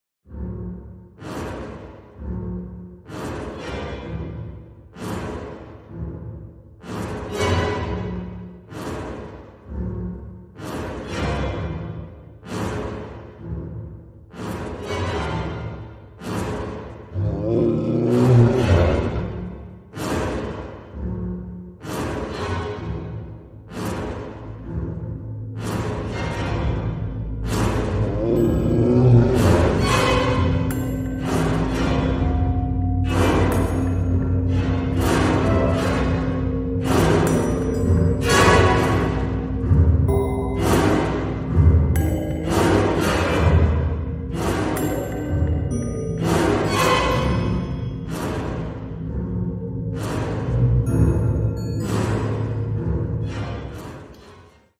Straight from the Dungeons of Hell!
We are doing a few of these 'life is good' videos...so must be a series...right?

You bet! With a video prompt and some manual effects...AI provides a stable of Dungeon masters and dwellers like you have never seen.